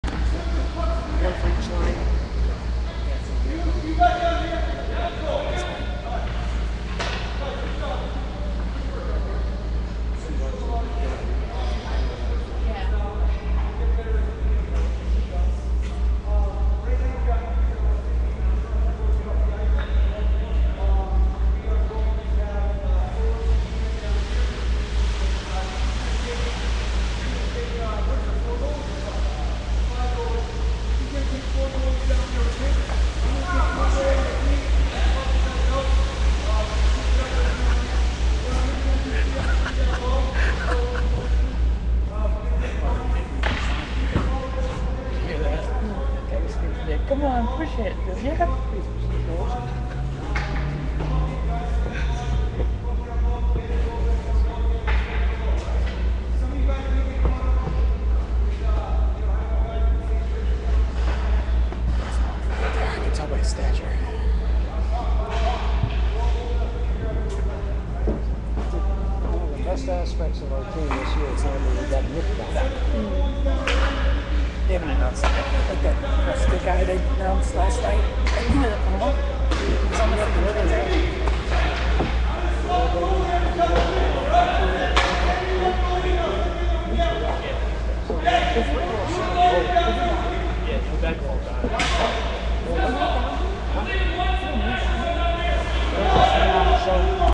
Wrap Up Group 2, June 2011
Wrap up after the second session. June 24, 2011 @ Danbury Ice Arena. Please note this is a public camp, and some bystander chat was captured. Some of the background dialogue may not be safe for work or young ears - Captured Live on Ustream